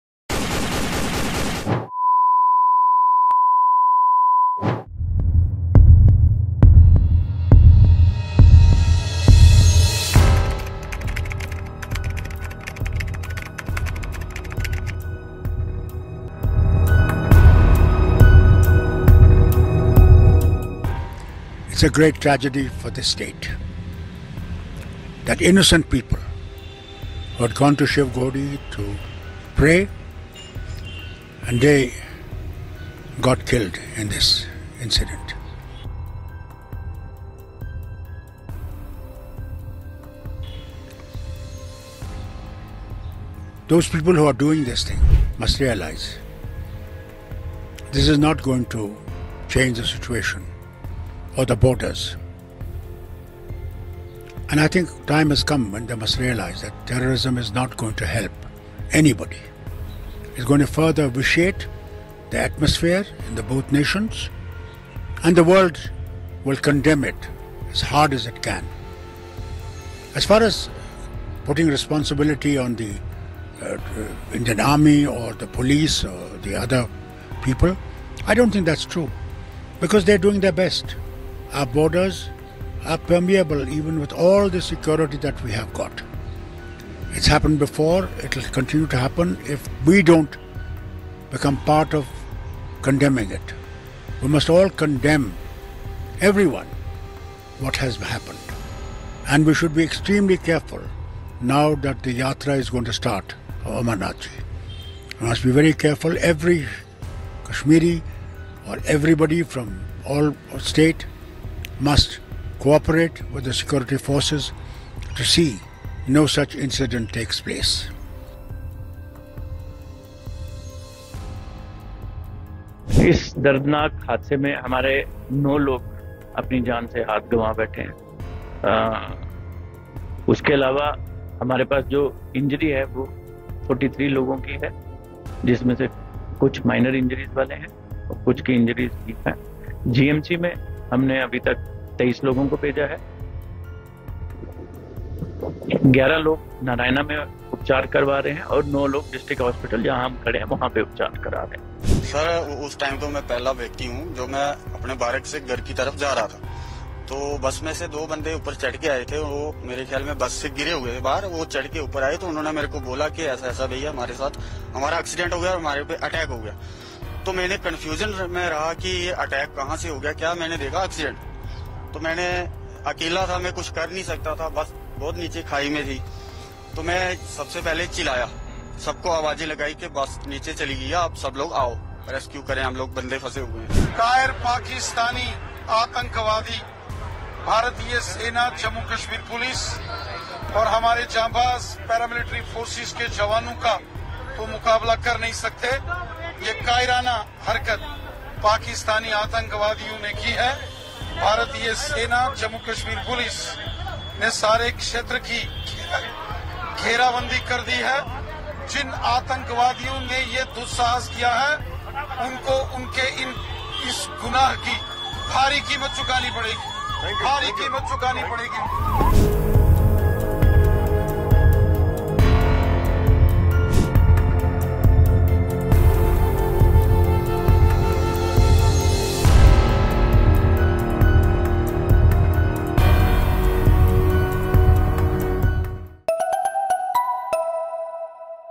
Reasi Terror Attack: Here's what happened and who said what after the attack
Terrorists attacked a bus carrying pilgrims from the Shiv Khori temple to Katra in Reasi, Jammu and Kashmir, on Sunday at 6:10 PM, causing it to veer into a gorge. The attack killed nine people, including a two-year-old, and injured 43 others. Victims are being treated in hospitals across Jammu and Reasi districts. J&K Lt Governor Manoj Sinha has announced ₹10 lakh ex-gratia for each victim's family and ₹50,000 for the injured.